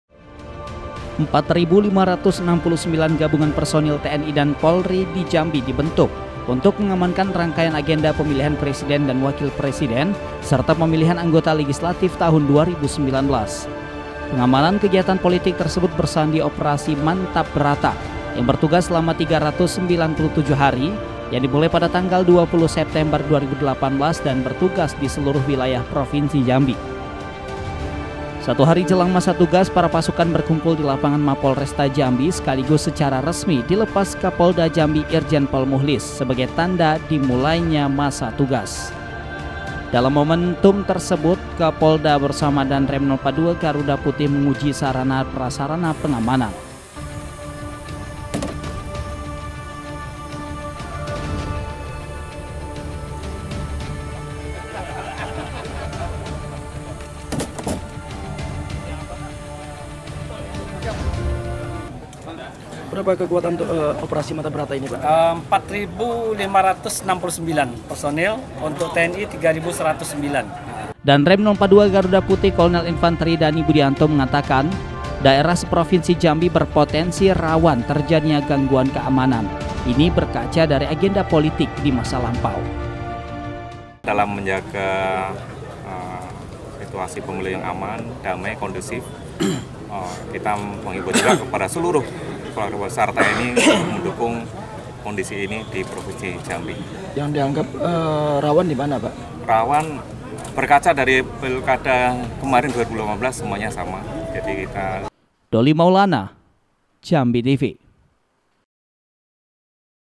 OPERASI MANTAP BRATA 2018 4569 PERSONEL GABUNGAN SIAGA AMANKAN PEMILU
OPERASI MANTAP BRATA 2018
4.569 gabungan personil tni dan polri di jambi dibentuk untuk mengamankan rangkaian agenda pemilihan presiden dan wakil presiden , serta pemilihan anggota legislatif tahun 2019 . pengamanan kegiatan politik tersebut bersandi operasi mantap brata , yang bertugas selama 397 hari  , yang dimulai pada tanggal 20 september 2018 , dan bertugas diseluruh wilayah provinsi jambi .